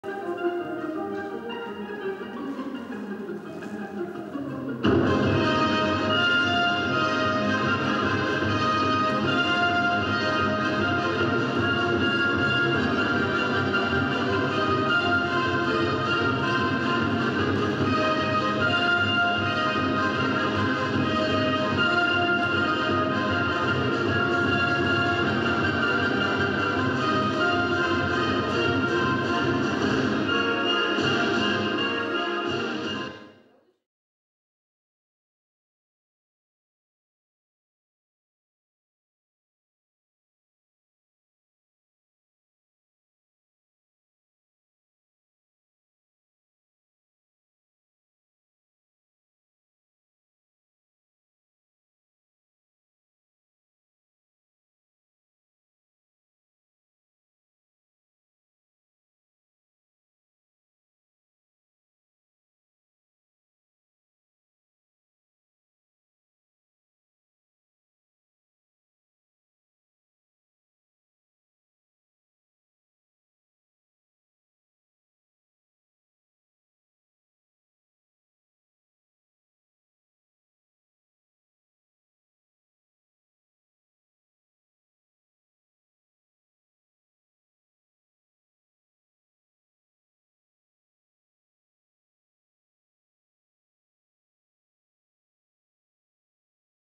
32 fouettes, all double. Maria, age 12 Russian ballerina.
Vladislav Kuramshin Russian ballet school.
Maria Bulanova, fouettes rehearsal Sertember 7th,
Fouettes teachers - Tatiana Petrova, Vladislav Kuramshin